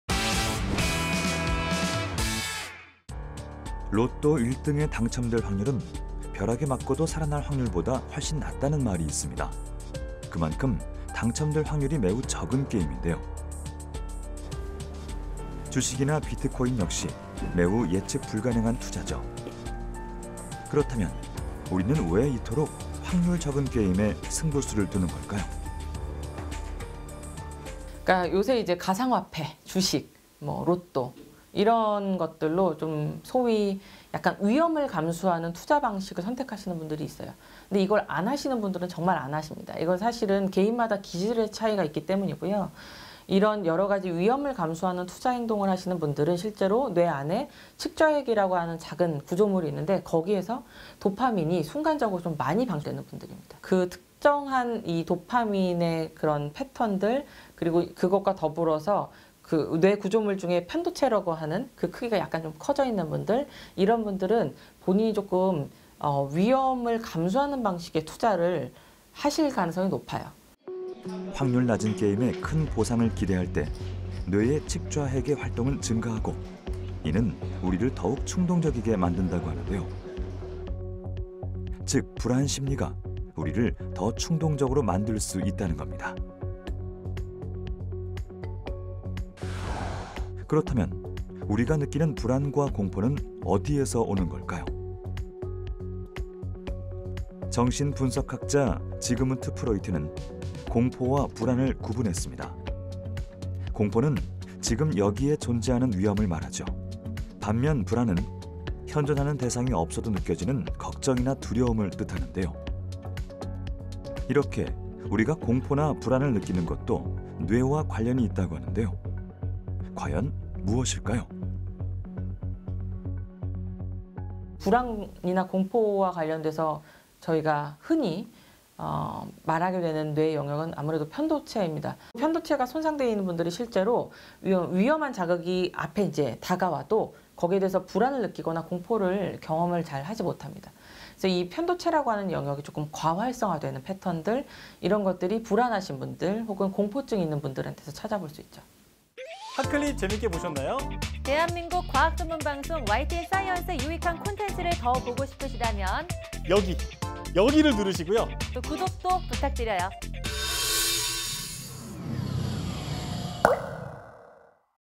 [핫클립] 위험을 감수하는 투자, 왜 그럴까? / YTN 사이언스
여러 가지 위험을 감수하는 투자 행동을 하는 사람들은
실제로 뇌의 측좌핵에서 도파민이 순간적으로 많이 분출된다고 한다.
또한 이런 사람들은 다른 사람들보다
편도체가 조금 더 크다고 한다.
그렇다면 불안과 공포는 무엇인지 알아본다.

위험을 감수하는 투자, 왜 그럴까?  / YTN 사이언스
[YTN 사이언스 기사원문]